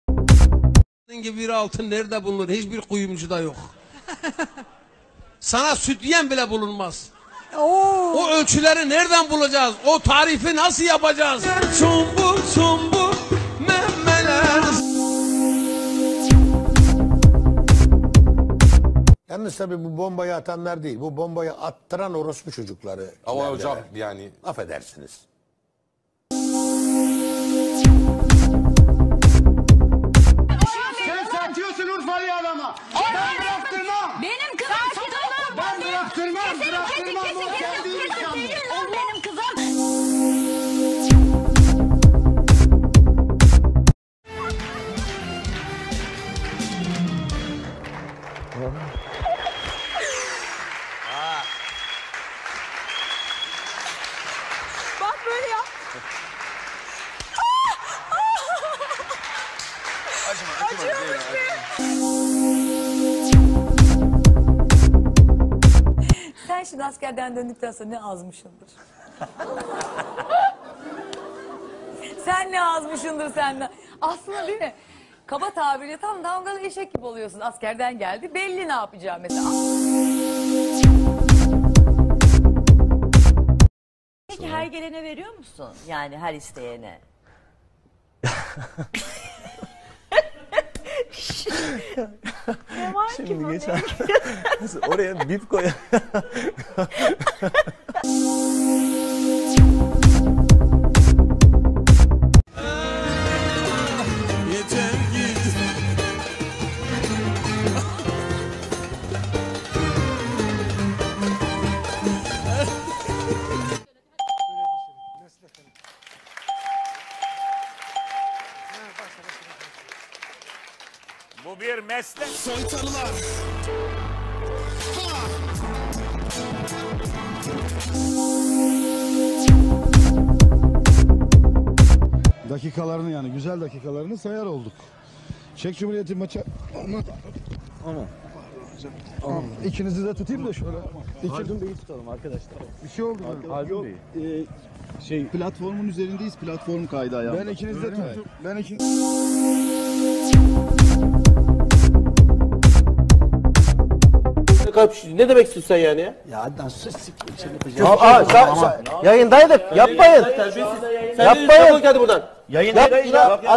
CANLI YAYINDA TACİZ OLAYLARI !
CANLI YAYINDA TACİZE UĞRAYAN
- HÜLYA AVŞAR
-ERMANHOCA
-İBOTATLISES
TV PRORGAMCILAR.